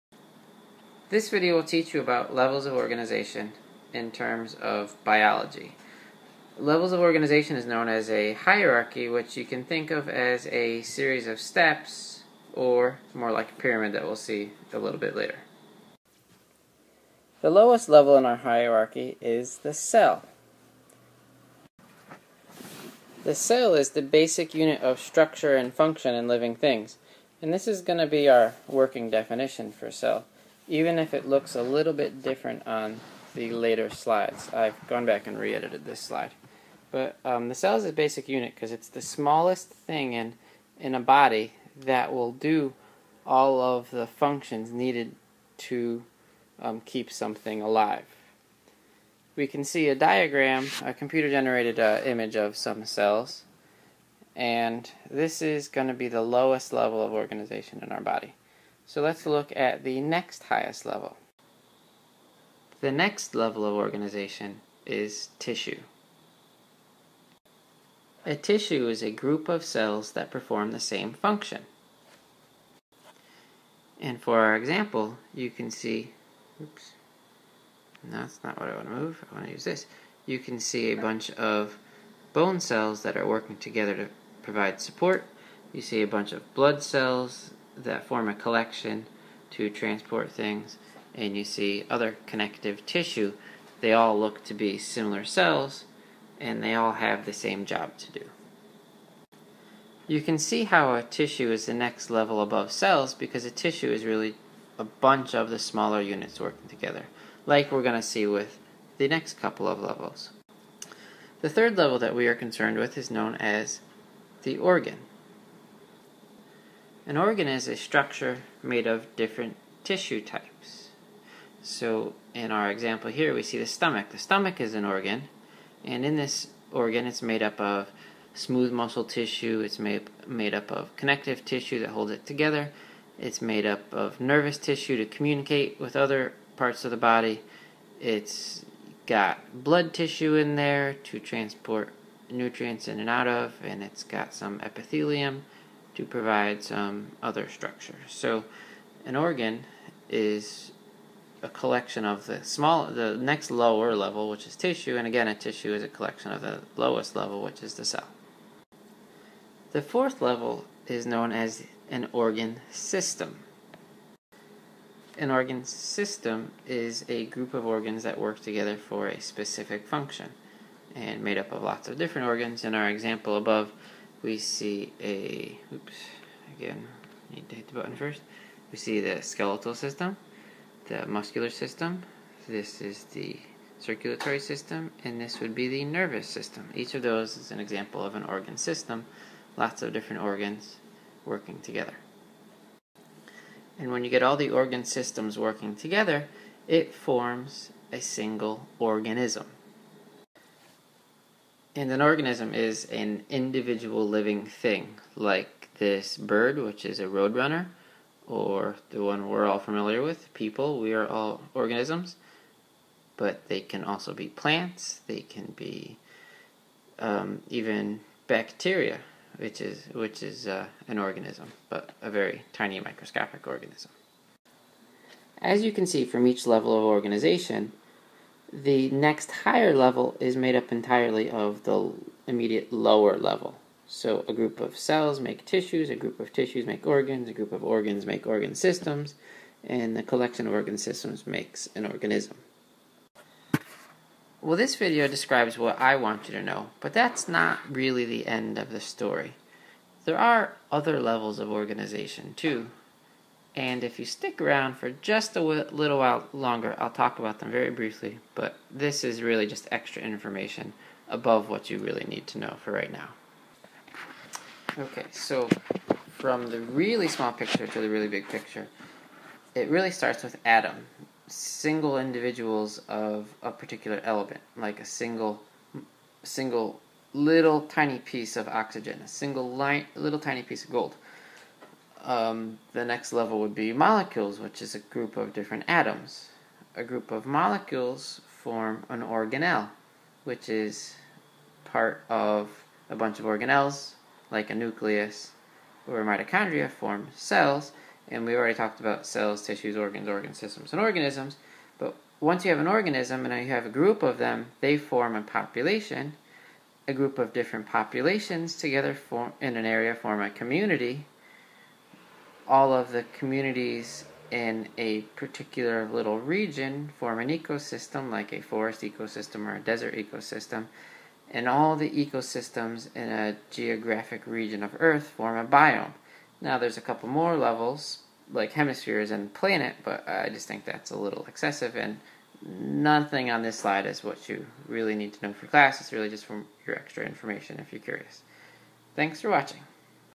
levels of organization
The biological levels of organization of living things arranged from the simplest to most complex are: organelle, cells, tissues, organs, organ systems, organisms, populations, communities, ecosystem, and biosphere.